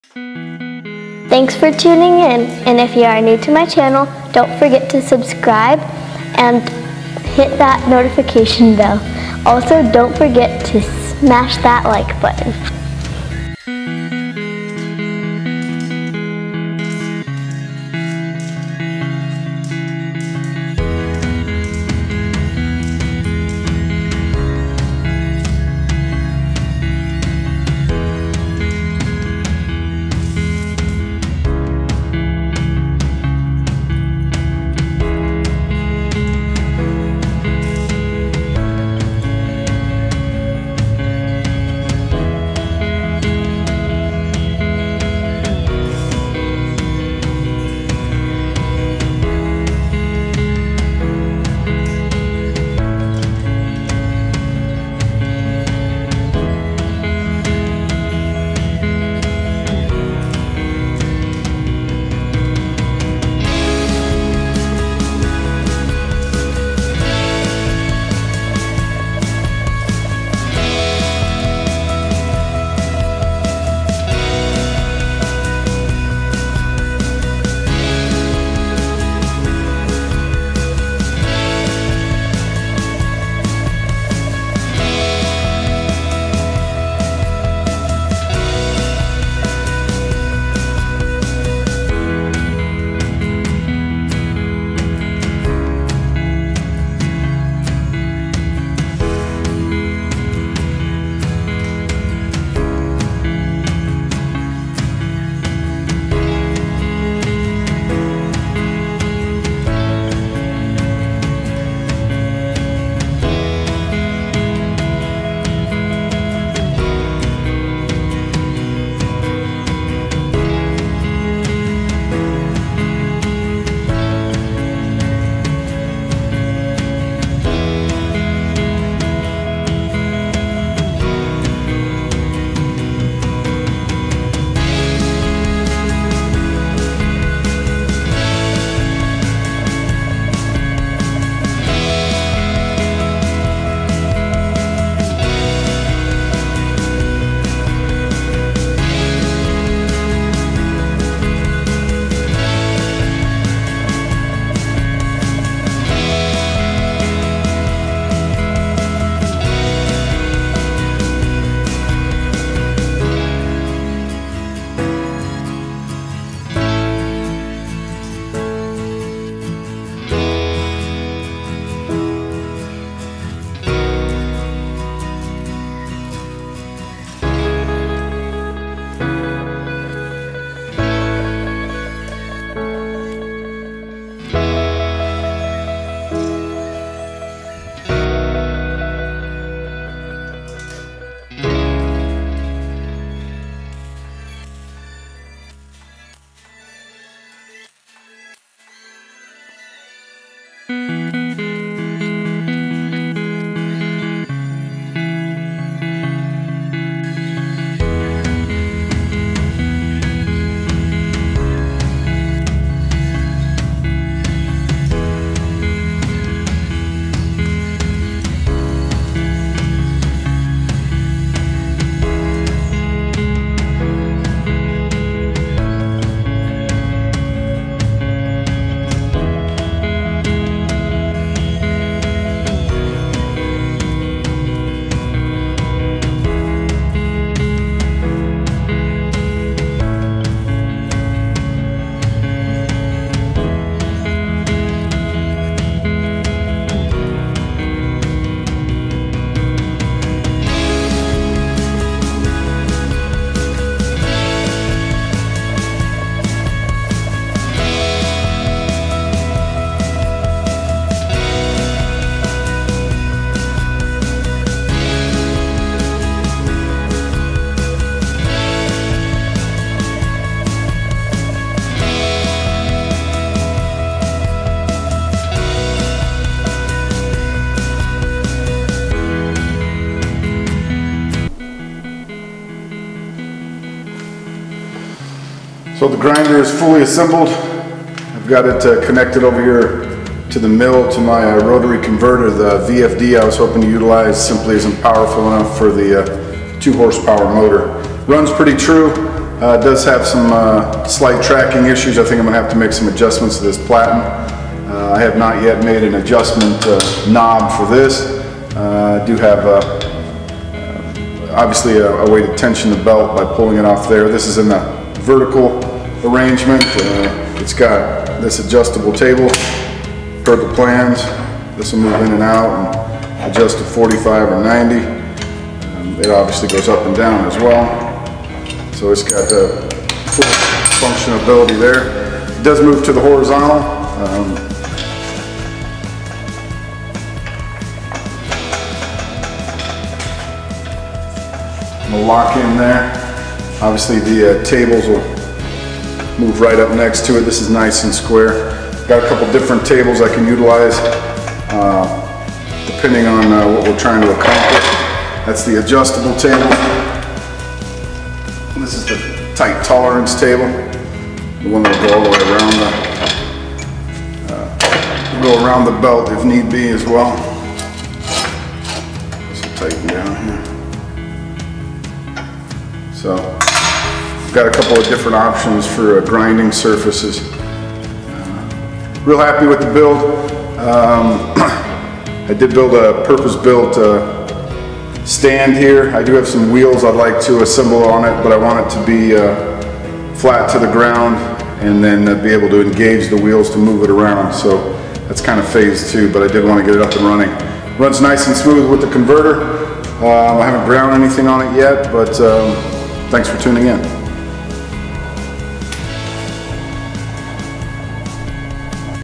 2x72 Belt grinder build (part 4)
Final assembly and paint as well as a purpose built stand.